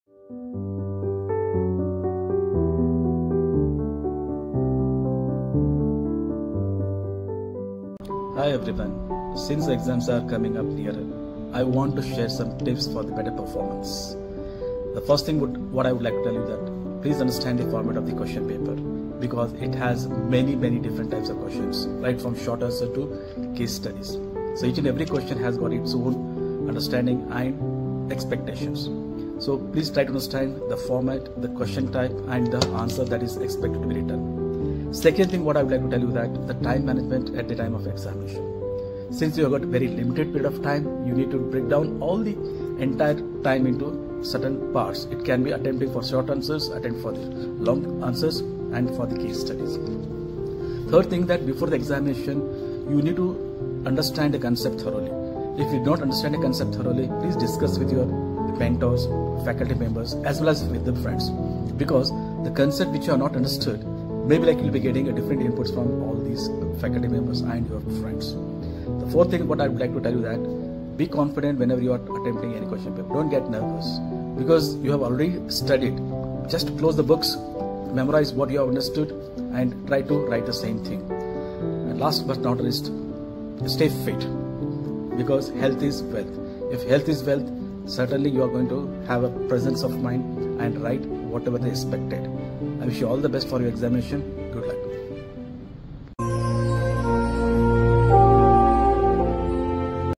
Exclusive Exam Tips from Dr. Girish Kulkarni: Boost Your Confidence & Score High
Exam stress? Don't worry! As always, our professors are here to support us. Dr. Girish Kulkarni shares exclusive tips to ace your exams.
Listen to Sir and boost your confidence and score high!